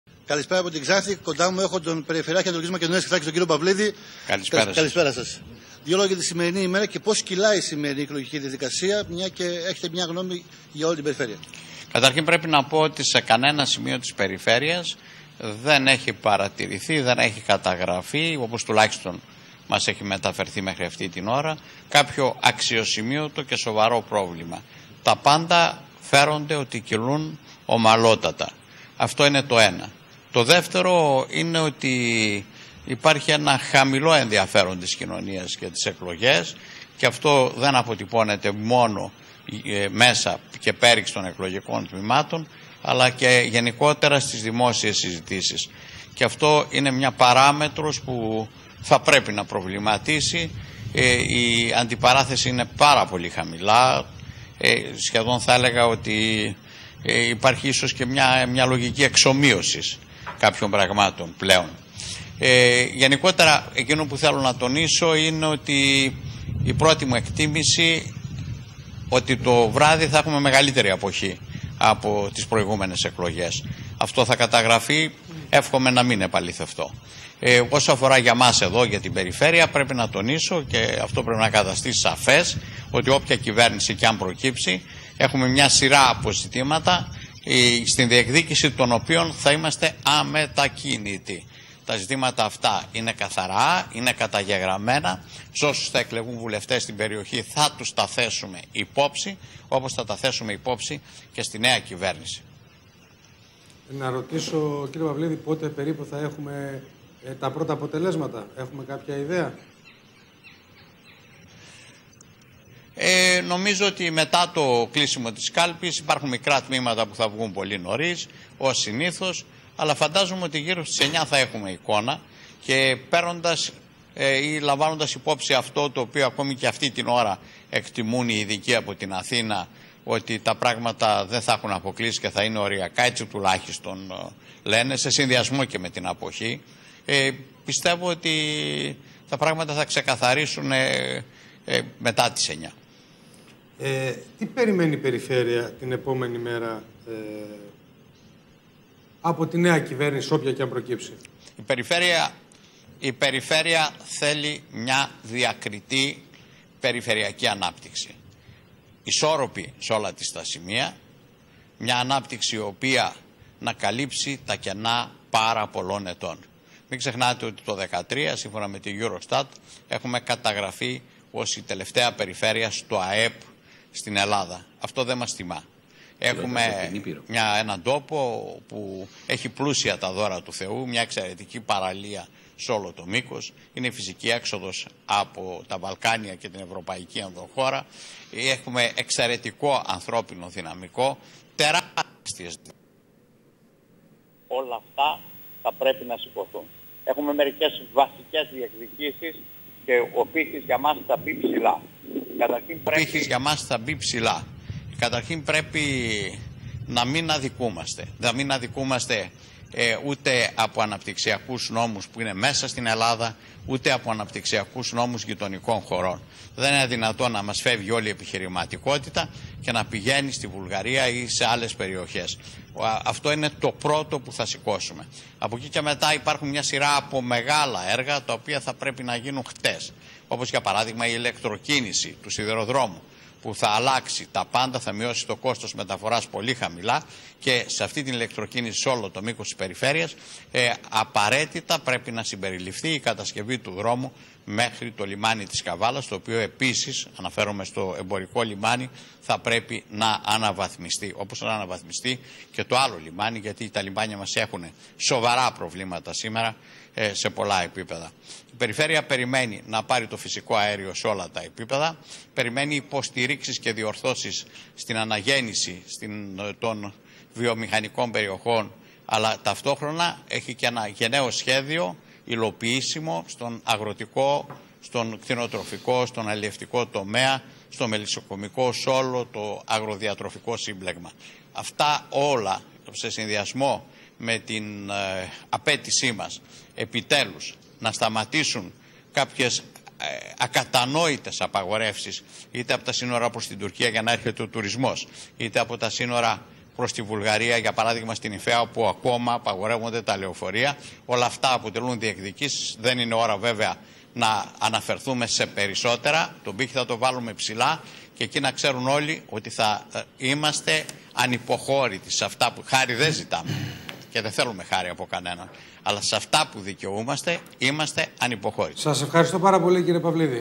Εκλογές Σεπτεμβρίου 2015 | Δηλωση Περιφερειάρχης Α.Μ.Θ. Γ. Παυλίδης
Δείτε τις Δηλώσεις του Περιφερειάρχη Α.Μ.Θ. Γεώργιου Παυλίδη